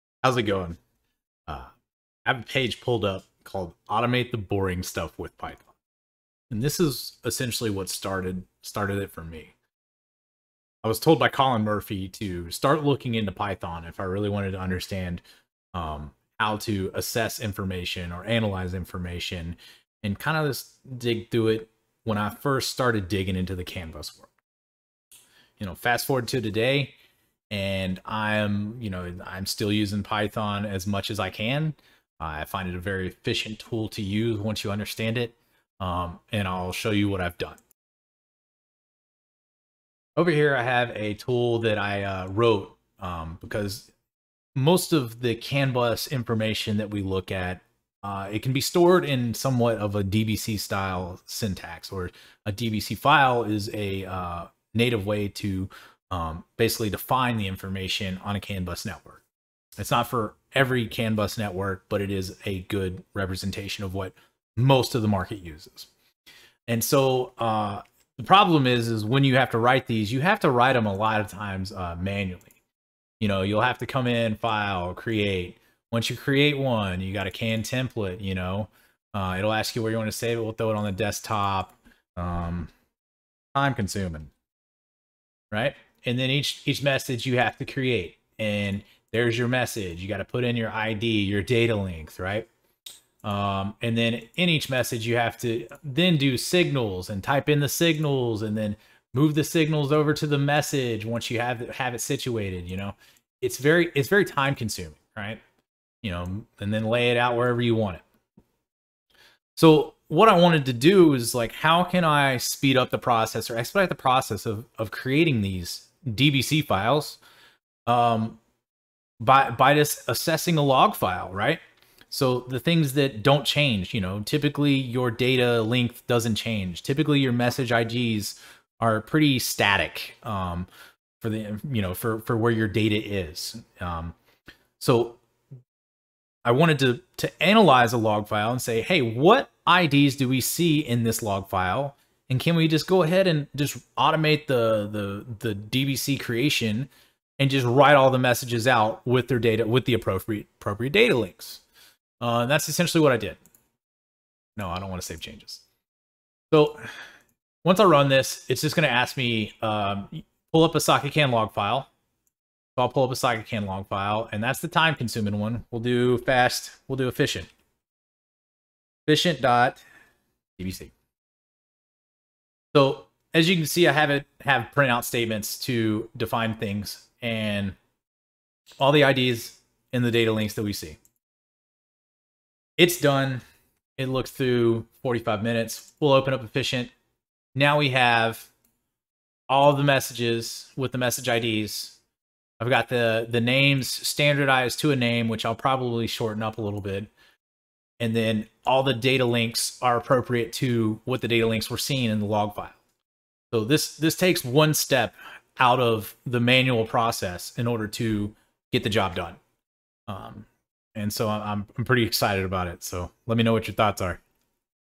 Automated DBC File Creation using Python
I Hate redundant work and am always looking for a better approach to solving problems. In this case, I was tired of creating DBC files and setting up the tedious tasks of the Message ID's and DLC's associated, so I used python to create a tool to do it for me. :)